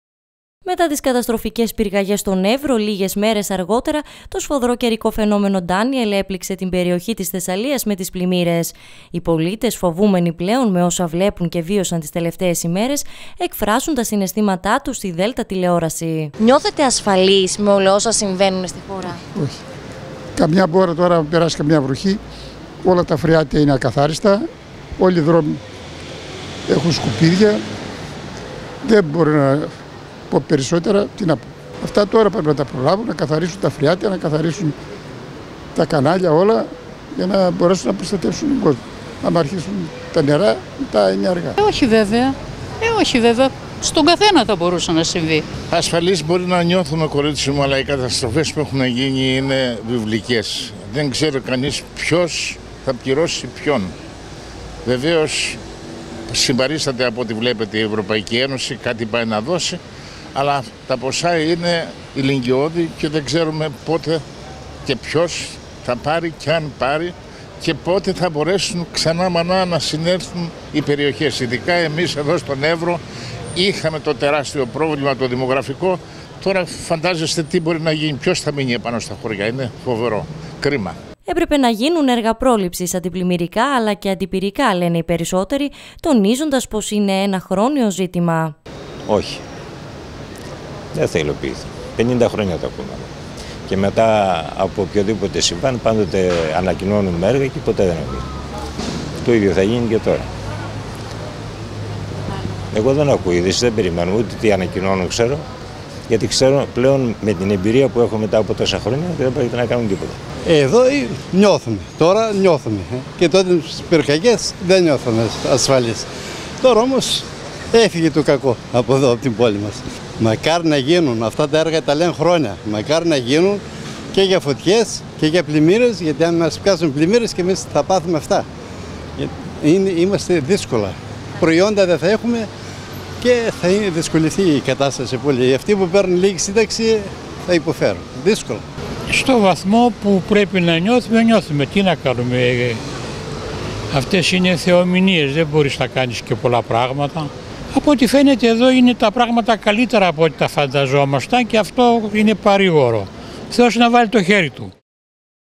Νιώθουν ασφαλείς οι πολίτες με τις καταστροφές που συμβαίνουν στη χώρα;
Πόσο ασφαλής νιώθουν τελικά οι πολίτες με τις καταστροφές που συμβαίνουν στην χώρα μας μέσα σε σύντομο χρονικό διάστημα όπως οι ανεξέλεγκτες φωτιές στη Θράκη αλλά και το σφοδρό καιρικό φαινόμενο Ντάνιελ με τις πλημμύρες στη Θεσσαλία; Οι πολίτες της Αλεξανδρούπολης εξέφρασαν τα συναισθήματά τους στην ΔΕΛΤΑ Τηλεόραση, απαντώντας στο ερώτημα.